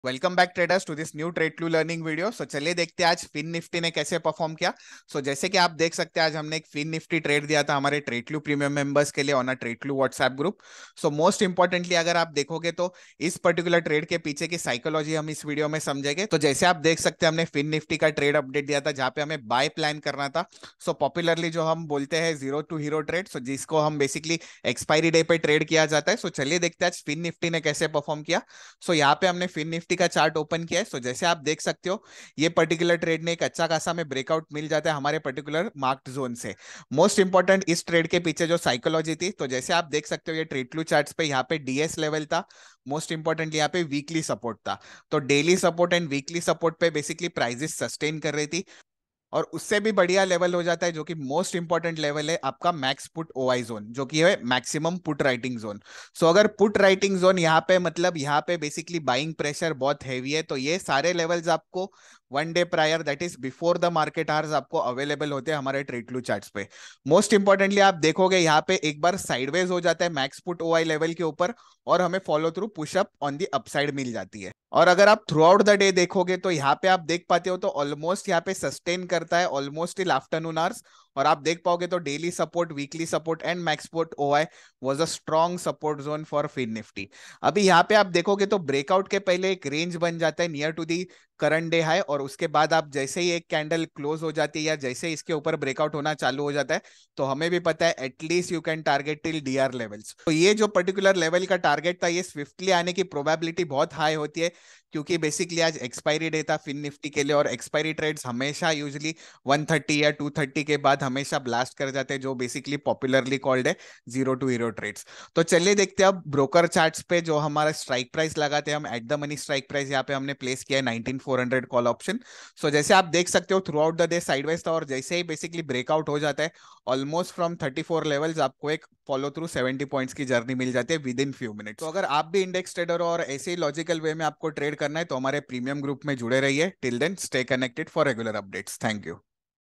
FINNIFTY: 20JUN2023 🎯🚀
In this video we are discussing Index trading using tradeclue levels and red -green dots.

For any assistance reach out to our support team.

SEBI REGISTERED ANALYST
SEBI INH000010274